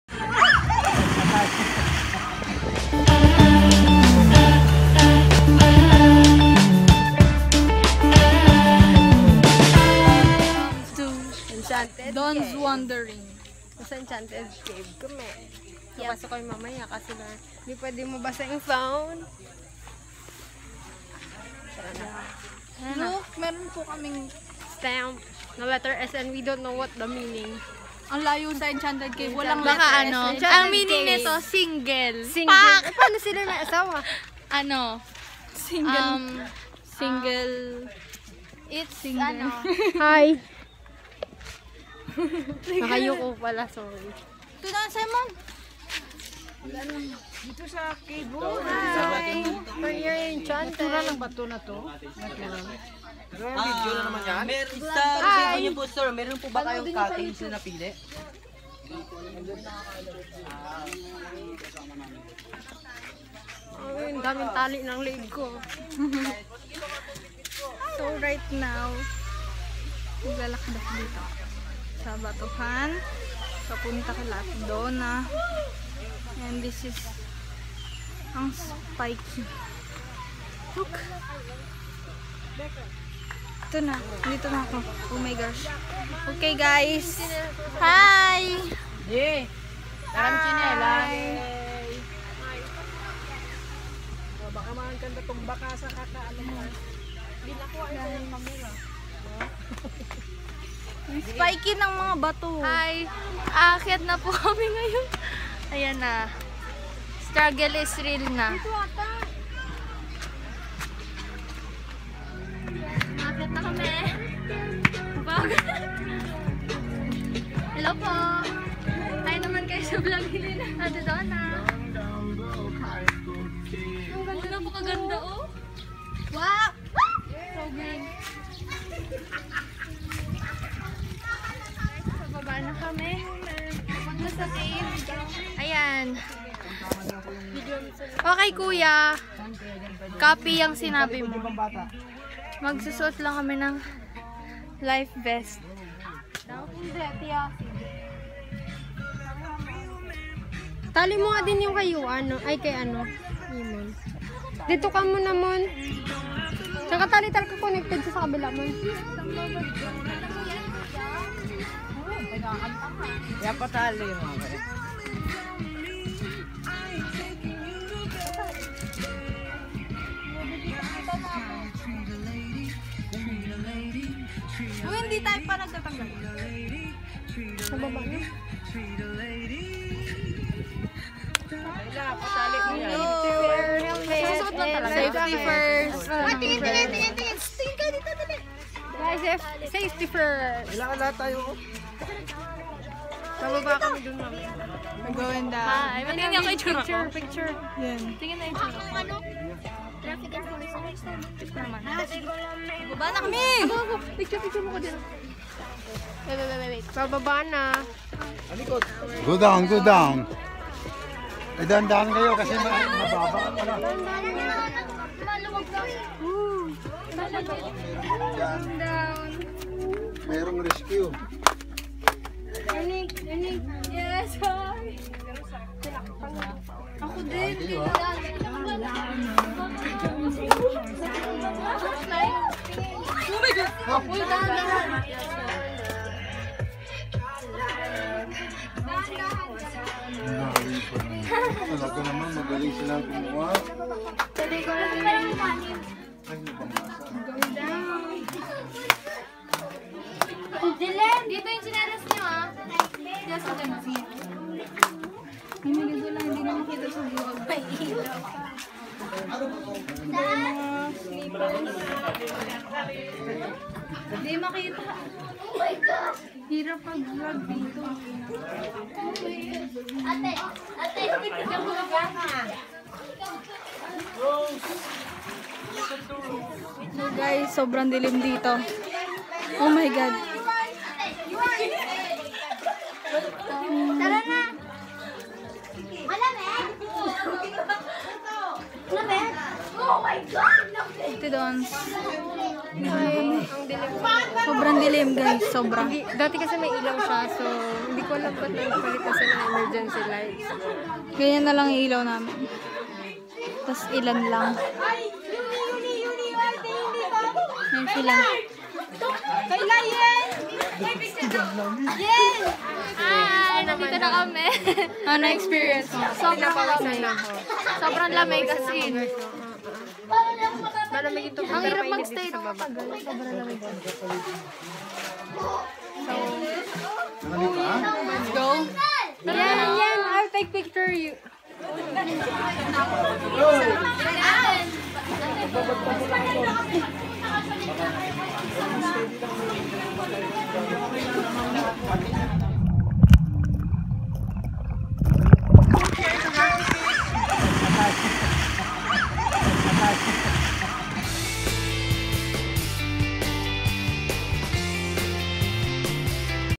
BOLINAO: ENCHANTED CAVE (Ft. My VITO FAMILY)
VLOG#31

BOLINAO: ENCHANTED CAVE

Entrance Fee
*Viewing+ Swimmimg - P200
*Viewing - P150

Enchanted Cave
-A Refreshing Underground Natural Spring Pool.
-30-45 minutes drive from Bolinao Town proper, a 15 minutes away to Patar White Sand.

Inside the cave is a bit dark, they change they lightning from electric to Solar Panel. Aside form the Cave they also have Pools for Kids (3-4 Feet) and for Adults (3-7 feet).

MUSIC in Backround is Played thru SPOTIFY Premium.
USER Name: 223336y3lrk522rmjl2i5un6y